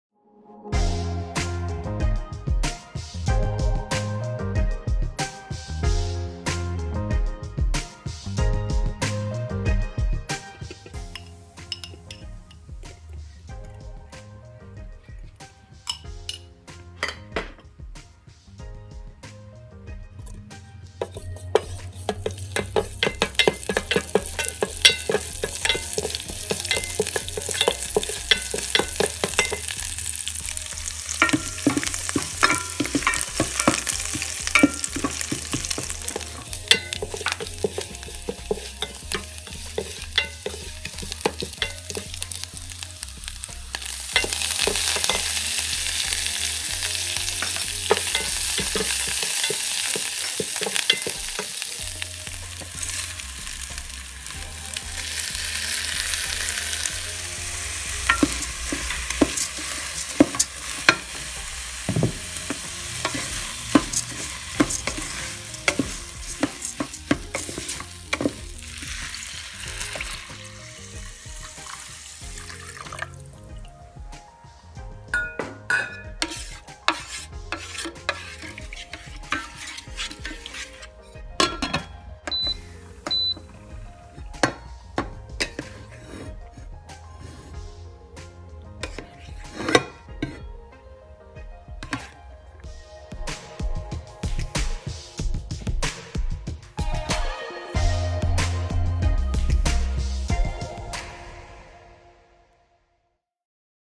JEERA Rice (Cumin Rice) Nelly’s Kitchen Home cooking
Jeera rice or Zeera rice is an Indian and Pakistani dish consisting of rice and cumin seeds. It is a popular dish in North India and Pakistan as an everyday rice dish. It is easy to prepare.
# JEERA Rice# cumin Rice Nelly’s Kitchen# Indian # simple Rice Dish